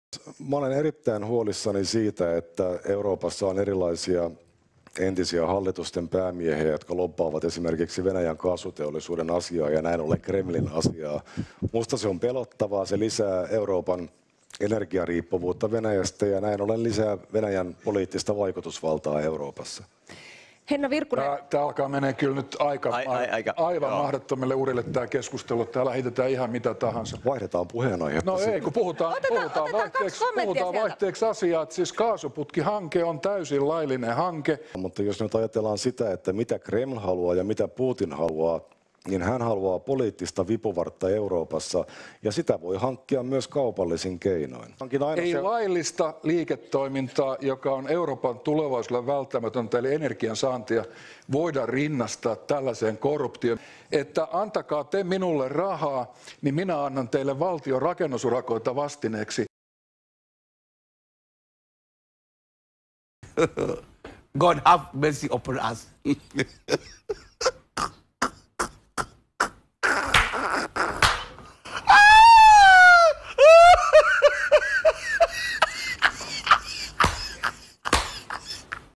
Eero Heinäluoma (SDP) kertoo kaasuputkesta 2019
Eero Heinäluoma (SDP) ja Jussi Halla-aho (PerusS) keskustelevat Venäjän kaasuputkesta toukokuussa 2019. Toinen näki Venäjän taka-ajatukset, toinen näki kivan hankkeen kivalta naapurilta. Viisi vuotta Krimin valtauksen jälkeen suuri osa Suomen poliitikoista söi Putinin kädestä ja taskusta.
Kaasuputki sabotaasi, Venäjän sota ja energiasota Eurooppaa vastaan. Mitähän seuraavaksi...